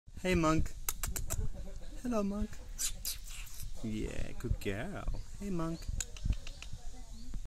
Hey Monk! Baan Unrak dog shelter resident Monk wagging her tail
I volunteered for a month and a half at Baan Unrak Thai Animal Sanctuary in Sanghklaburi in Thailand.

Here, I'm in the pen with Monk. She's feeling too lazy to get up and socialise with me, but she wags her tail every time I call her name